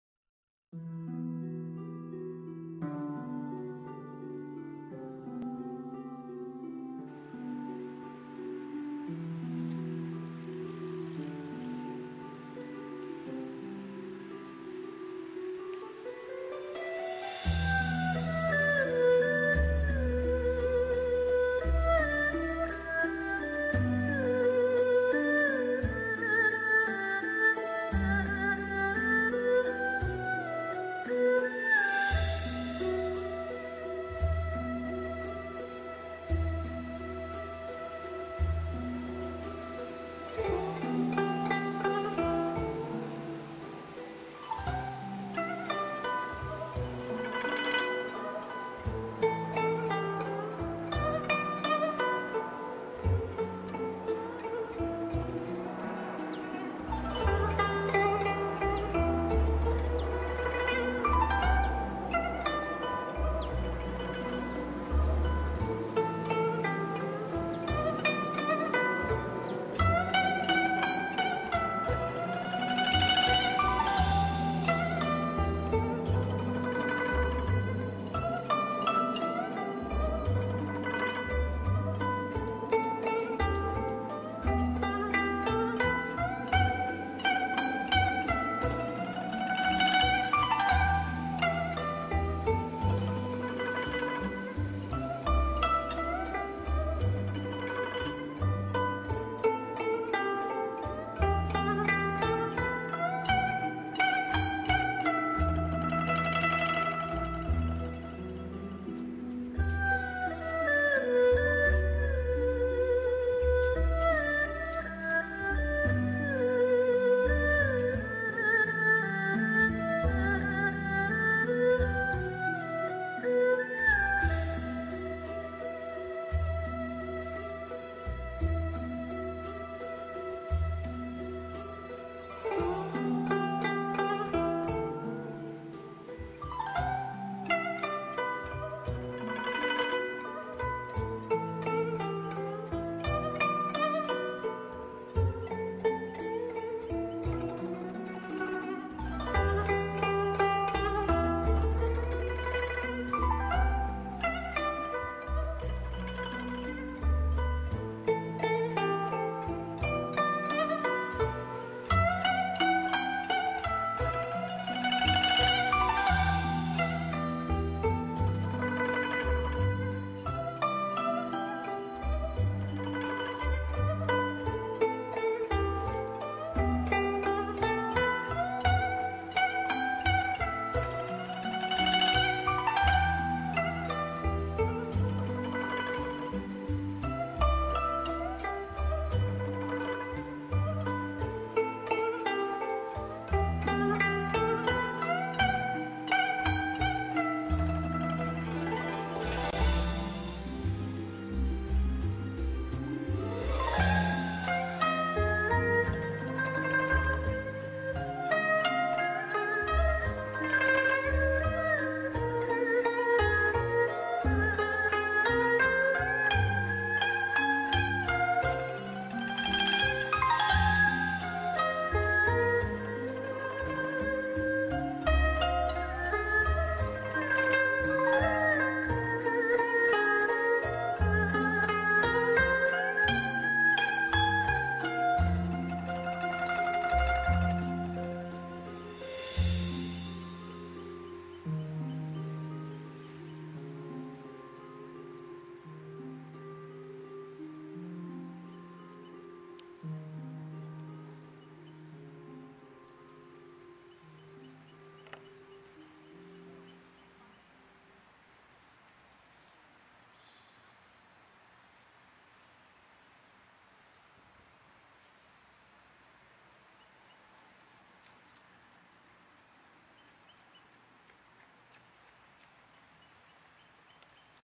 NEW ! a Beautiful Guzheng & Erhu Chinese Music for TAIJI Sword !
a beautiful Chinese New Age music played with  GUZHENG as the focus instrument , the amazing sounds of ERHU for the chorus part , and the piano accompanying the whole music..giving the music a touch of NEW AGE !